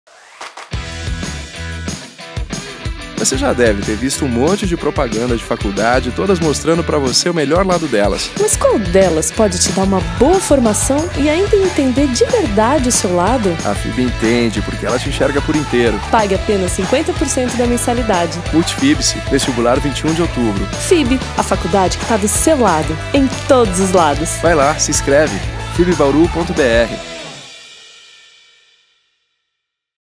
Vestibular FIB 2018 - Inscreva-se!
Você já deve ter visto um monte de propaganda de
faculdade, todas mostrando pra você o melhor lado delas.
Mas, qual delas pode te dar uma boa formação e
ainda entender, de verdade, o seu lado?
A FIB entende porque ela te enxerga por inteiro.
Pague apenas 50% na mensalidade*.
Multifib-se.
Vestibular 21 de Outubro
FIB, a faculdade que está do seu lado.
Em todos os lados.
Vai lá, se inscreve agora.